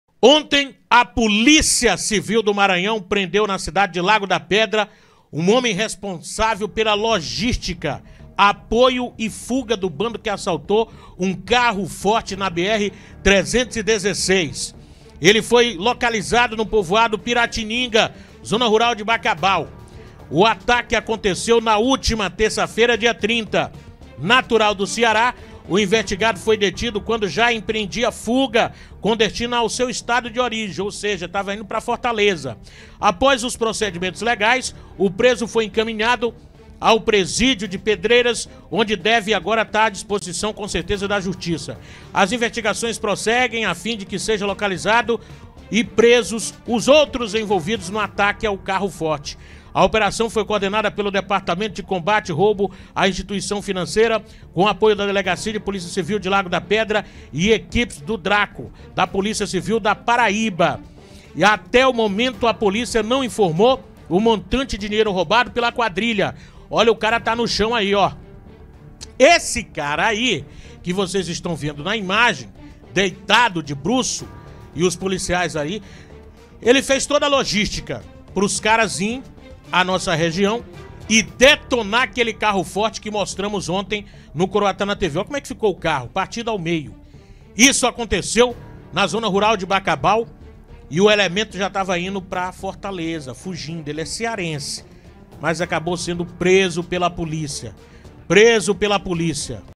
HOMEM QUE PARTICIPOU DO ATAQUE AO CARRO FORTE É PRESO EM BACABAL
ANTONIO SILVA
ELTON ANTONIO
LUCAS RAMOS